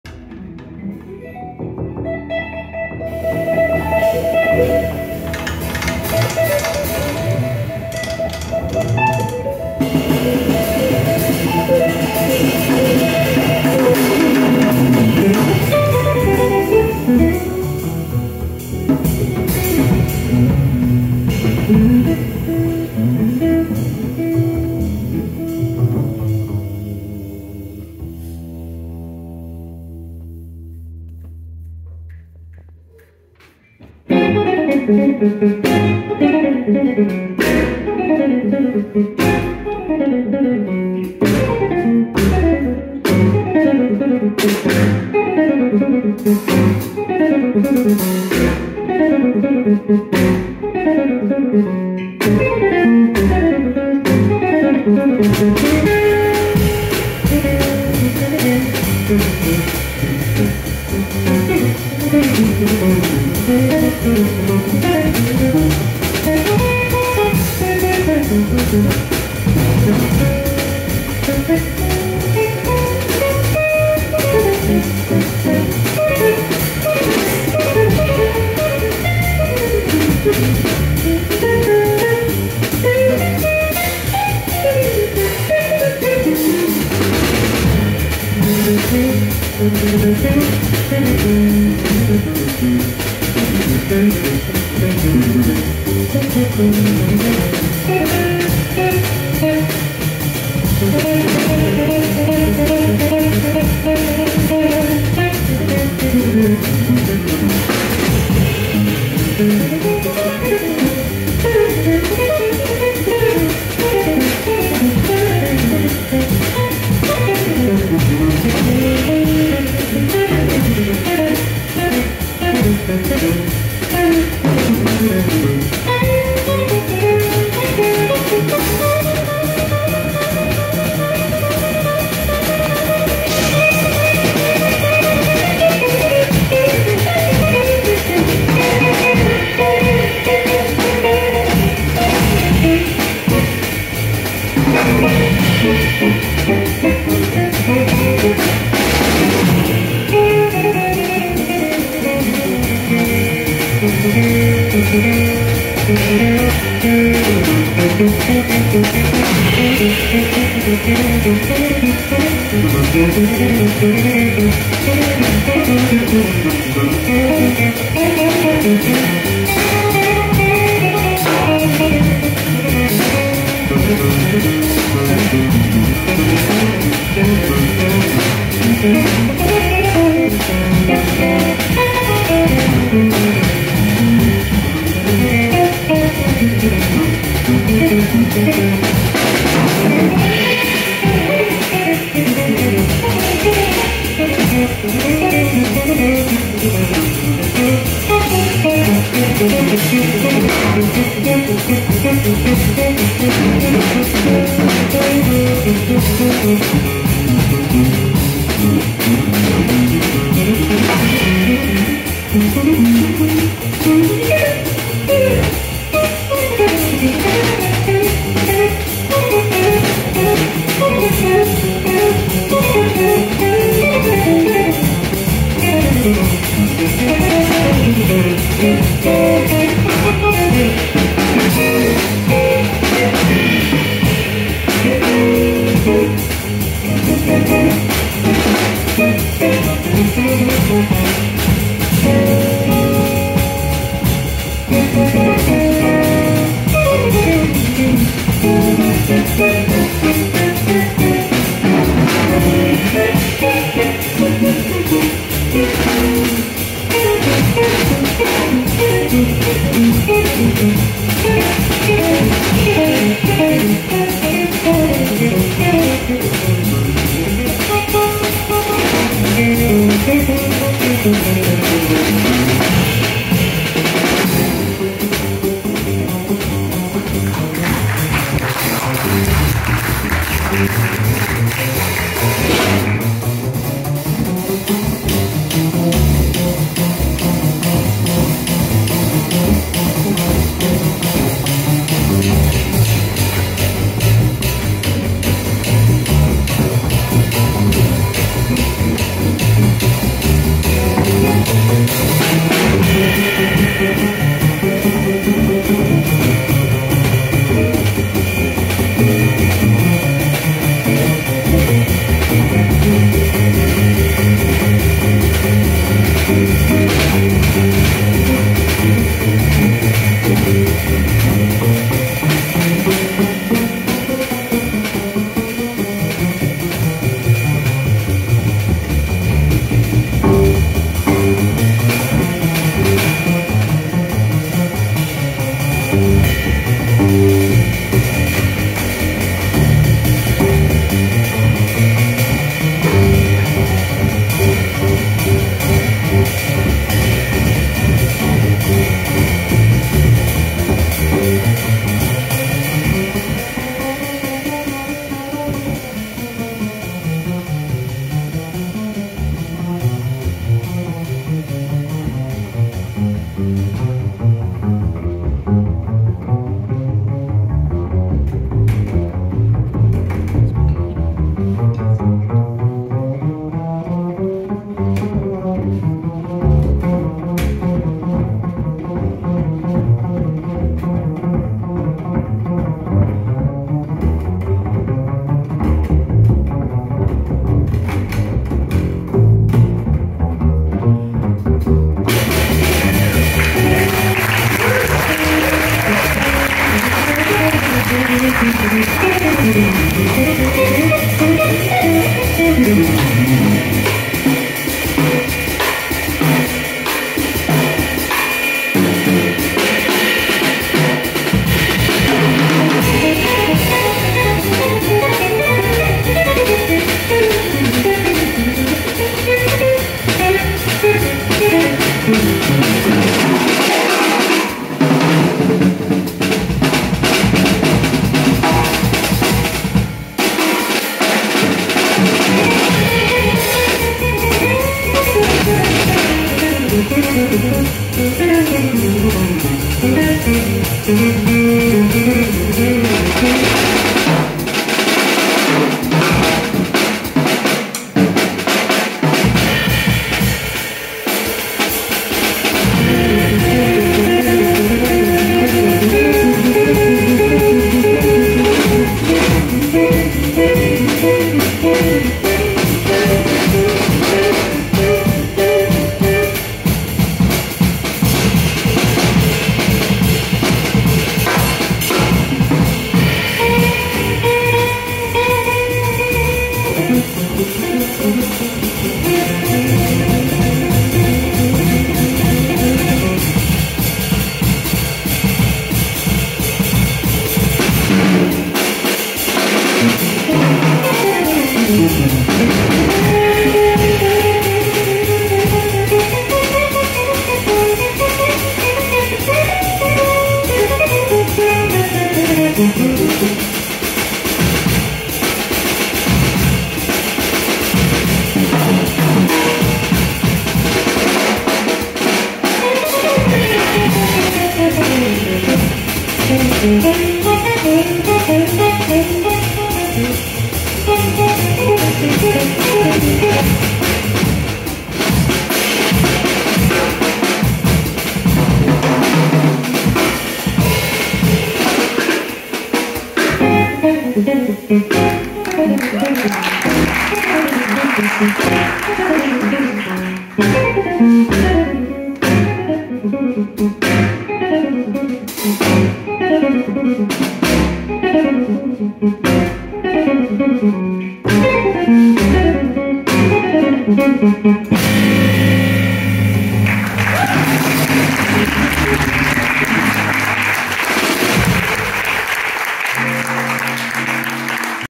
The James Zito Trio (Aug. 27, 2022)
‎@James Zito  with the James Zito Trio playing live at ‎@Timucua Arts Foundation  for the Guitar Series concerts

Personnel:
James Zito, Guitar
Thomas Milovac, Bass
Gabriel Landa, Drums